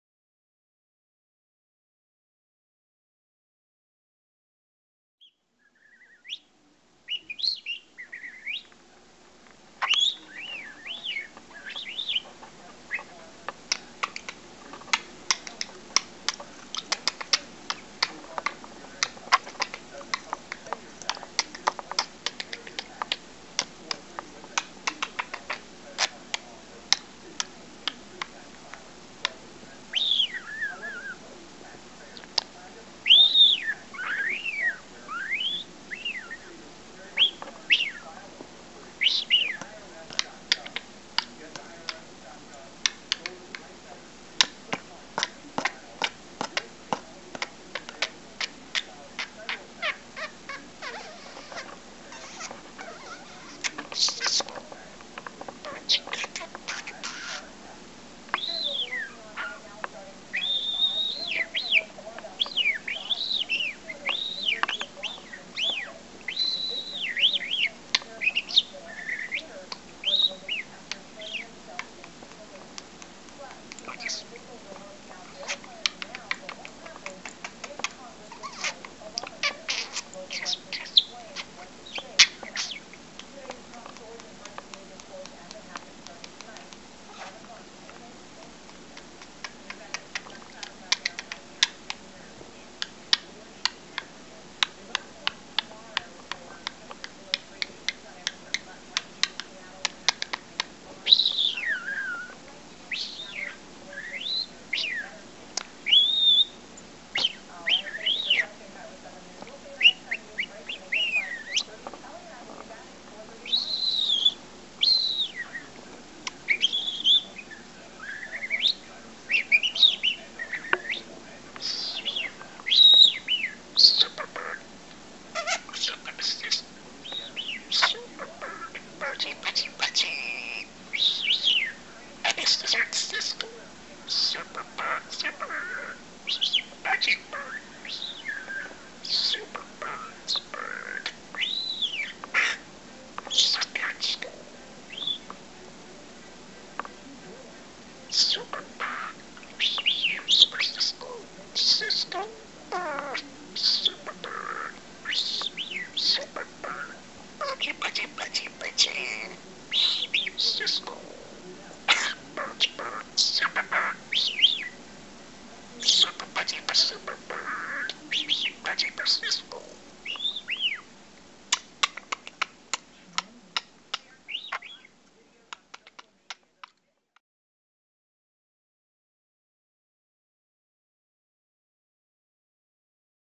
Cisco Talking
Cisco started to talk about two weeks after I got him, and he was only about 2-1/2 months old at that time.  He's now 5 months old and is talking a lot.  He knows about 20~25 words so far, and a bunch of different whistles that I've taught him.  He even makes up his own little phrases sometimes with the words he knows.  This is the first video of him talking.